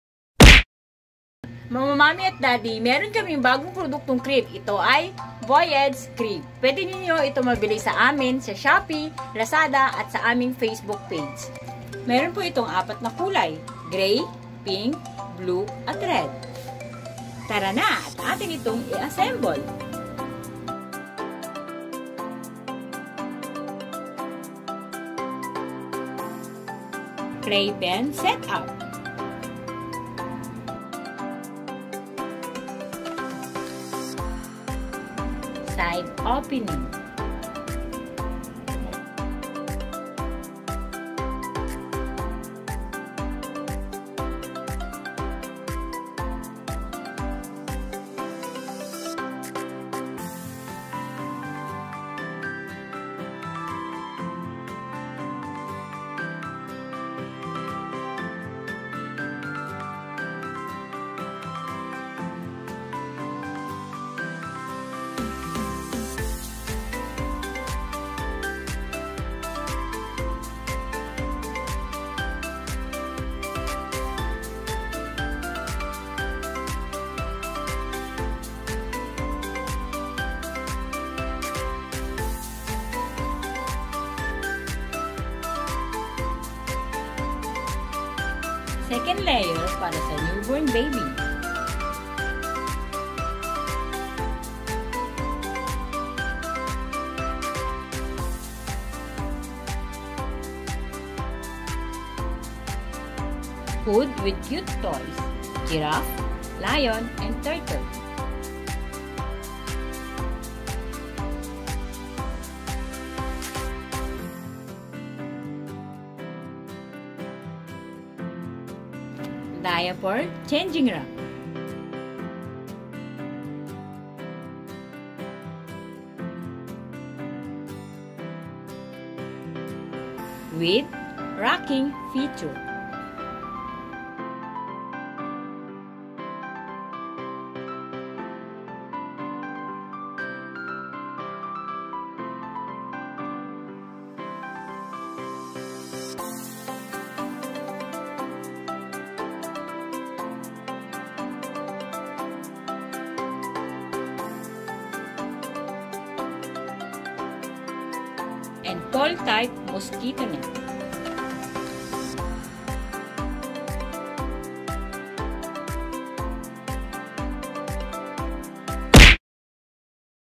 VOYAGE Crib I Quick Review by Little Feet
Quick Review of VOYAGE Crib for LAZADA or Shopee buyers

You can buy Voyage Crib at

Colors: Gray, Blue, Pink, Red

Features:
-With Rocking
-Diaper changing Rack
- Hanging Toys
-Dome Type hood with Toys
-Pole Tyoe Mosquito Net
-Crib to playpen